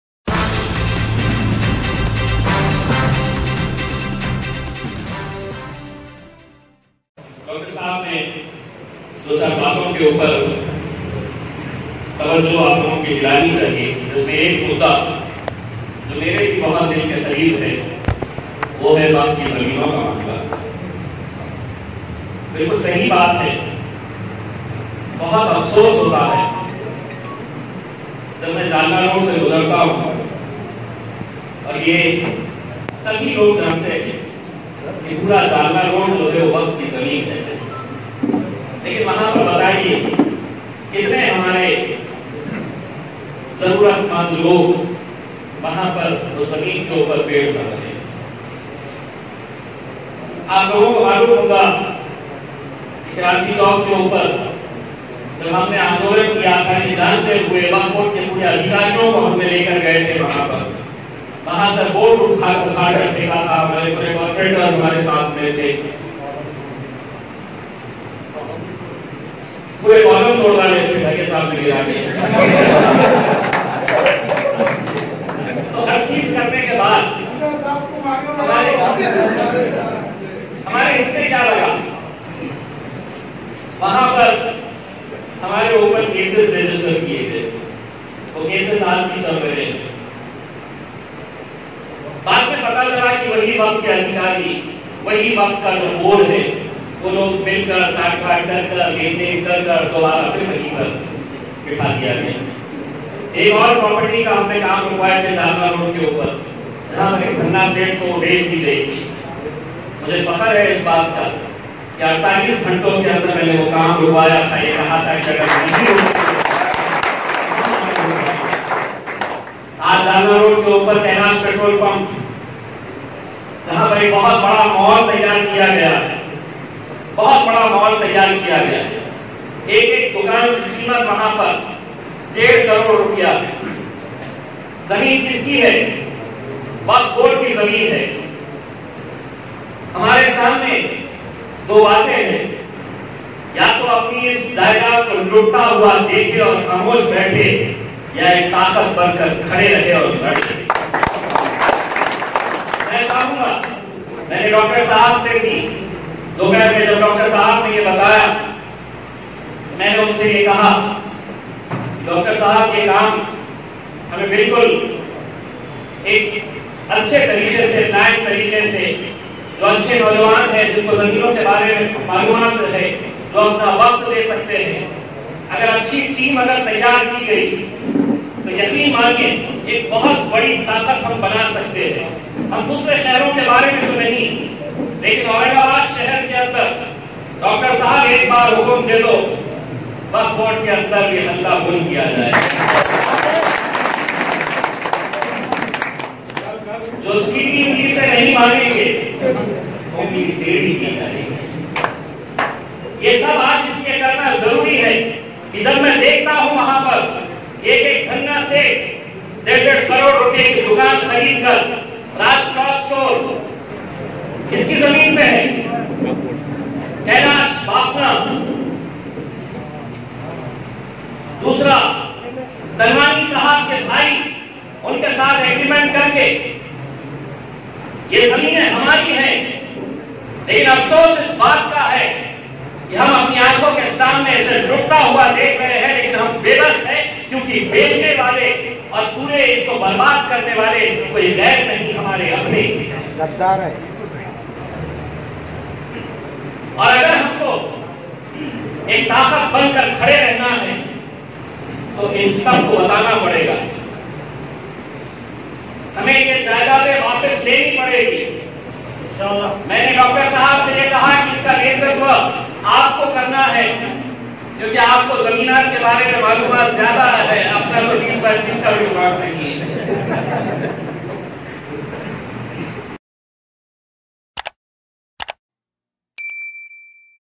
वक़्फ़ बोर्ड की ज़मीनो के लिए इम्तियाज़ जलील ने दिया आंदोलन का इशारा!!
Mp.Imtiaz Jaleel sahab ne waqf board ki zamino ke liye lockdown ke baad aandolan ka diya ishaara, bahot jald ek team bana kar is mudde par ladegi AIMIM, 21 August 2020 ko party ke ek program me kahi ye baat
(City Reporter: Furquan Ahmed)
       [Marathwada Mahiti]